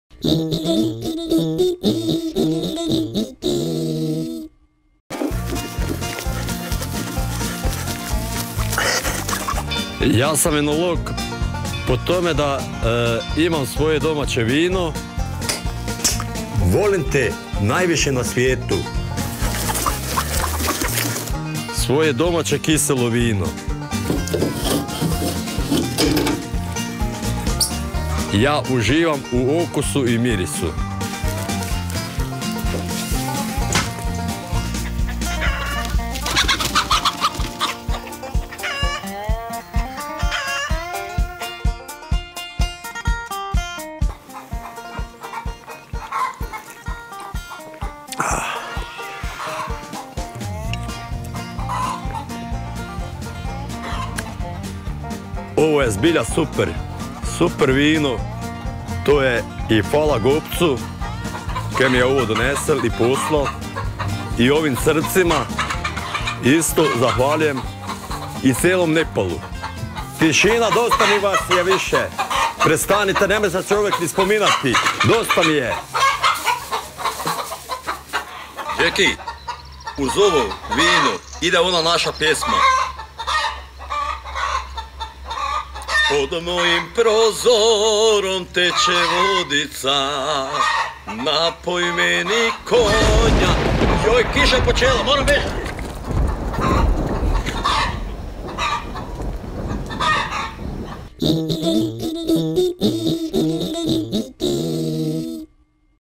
Enolog Rajko - recenzija vina Davora Gobca s Himalaje
Naš dragi, nažalost pokojni prijatelj Radovan, u serijalu "Koledžicom po svijetu" pokazao je svim svjetskim enolozima, što misli iskusni i iskreni znalac iz Donjeg Dragonošca!